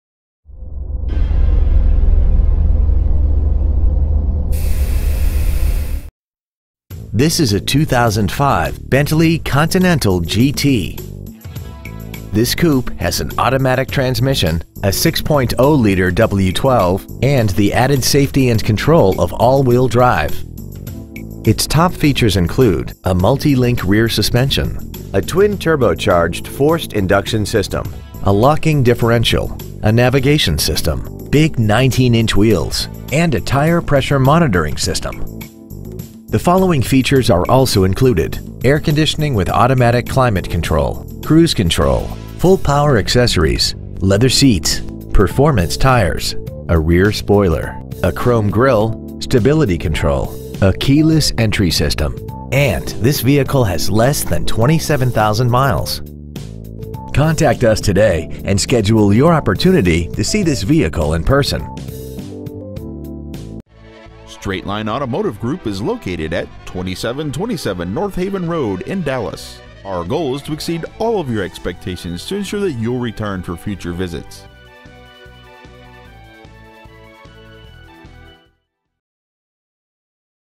Pre-Owned 2005 Bentley Continental GT Dallas TX
We are proud to present this 2005 Bentley Continental GT .

 Year : 2005
 Make : Bentley
 Model : Continental GT
 Engine : 6.0L DOHC 48-valve EFI twin-turbocharged W12 engine
 Trans . : Automatic
 Exterior : Black
 Miles : 26,375
 Interior : Tan